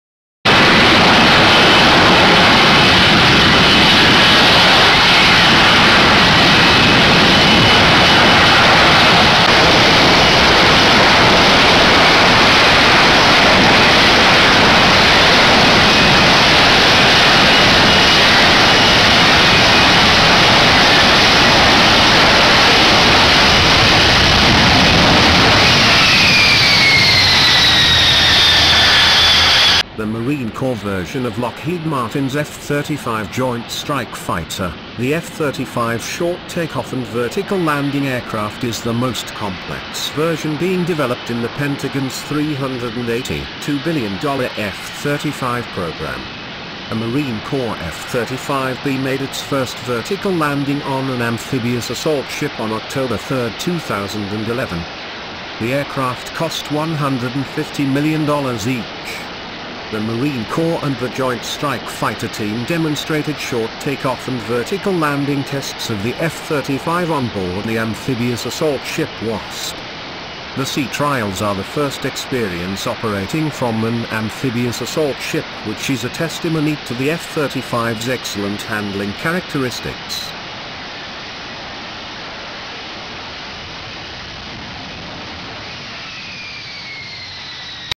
F-35 Lightning Makes First Vertical Landing on the Marine Corps Amphibious Assault Ship USS Wasp
- The Marine Corps version of Lockheed Martins F-35 Joint Strike Fighter, the F-35B short-takeoff and vertical-landing aircraft is the most complex version being developed in the Pentagon's $382 billion F-35 program.
A Marine Corps F-35B made its first vertical landing on an amphibious assault ship on Oct. 3, 2011.
The aircraft cost $150 mil each.
The Marine Corps and the Joint Strike Fighter team demonstrated short take-off and vertical landing tests of the F-35B Lightning II onboard the amphibious assault ship Wasp.
The sea trials are the first experience operating from an amphibious assault ship which is a testimony to the F-35B's excellent handling characteristics.